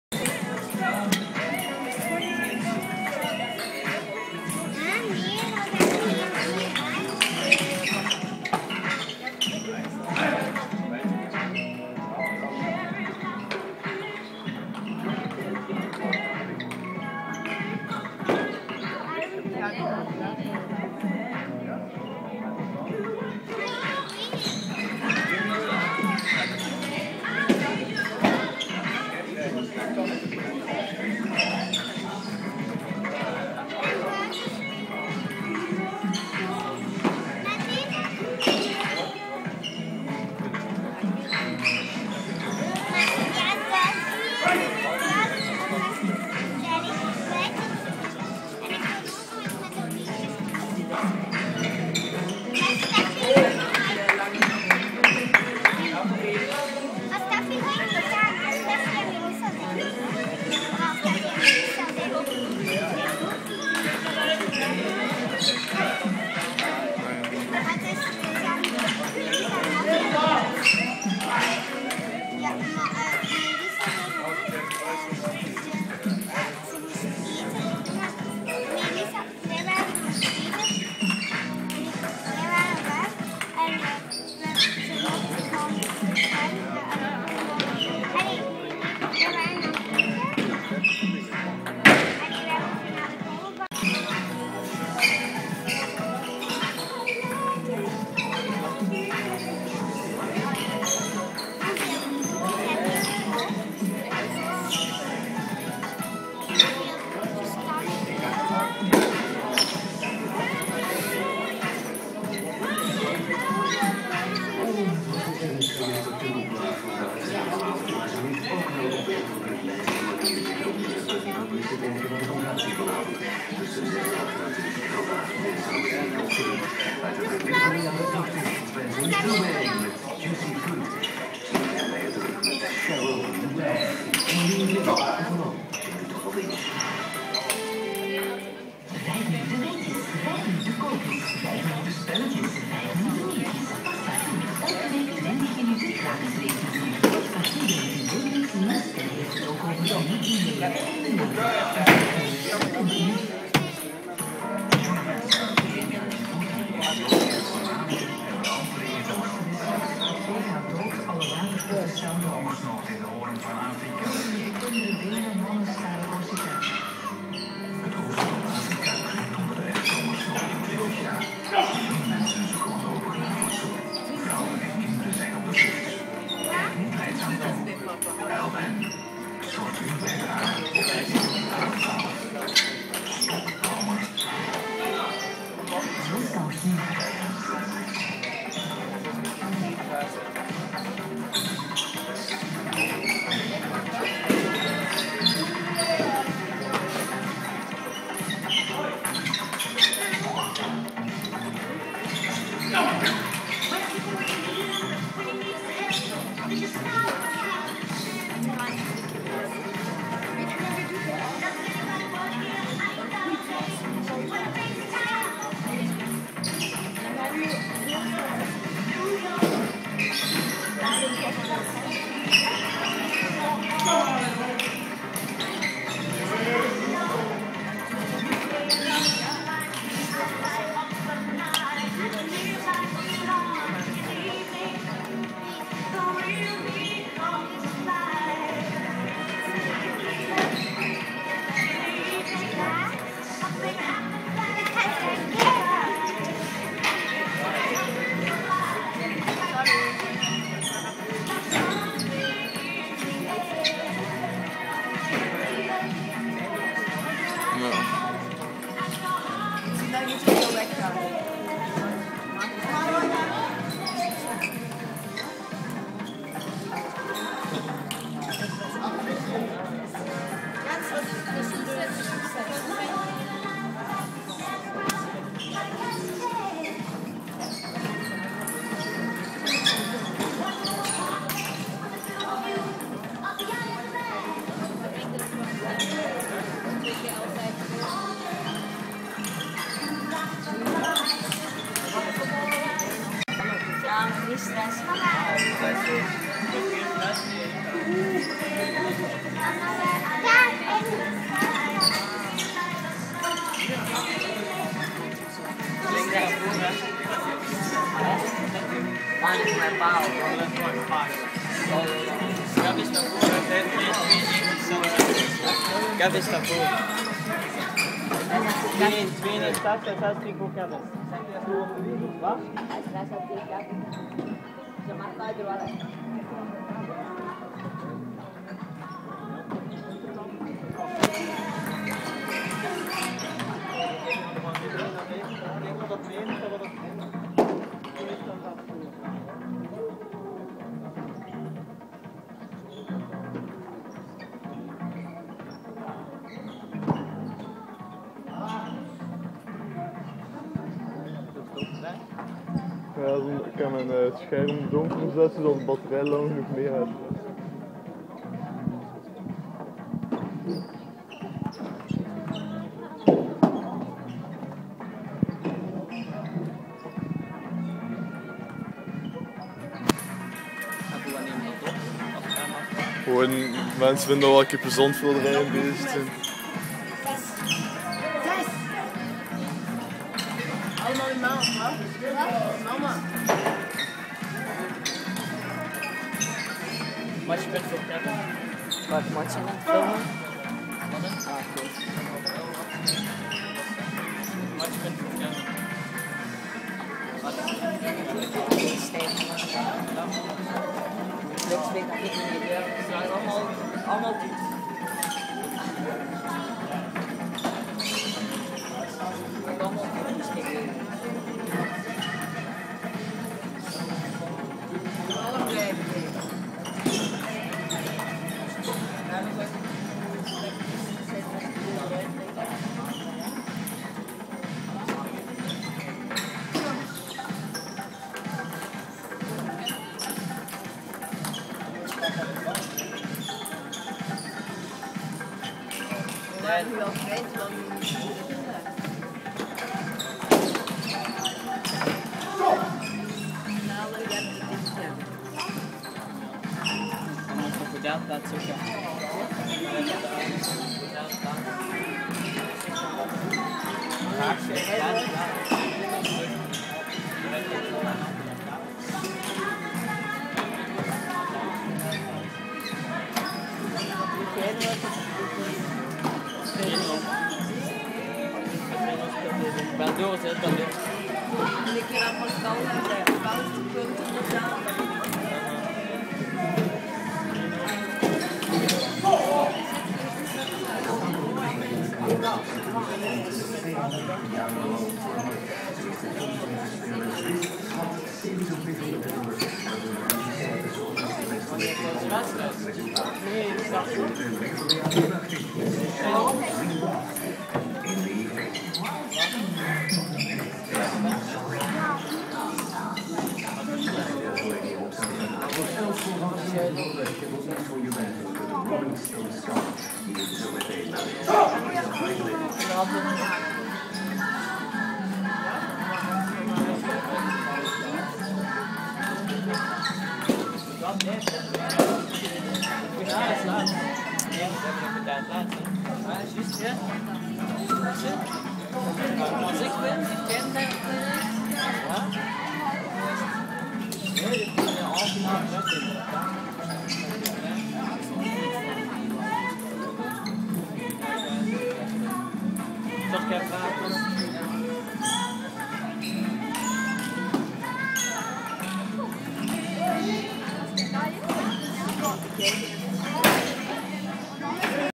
Jens Versluys (C2) - Nic Bockaert (B2): 3-in-1 Rooigem 2011
Halve finale 3-in-1 tornooi TTC Rooigem in de afdeling competitie.

Jens Versluys C2
Nick Bocaert B2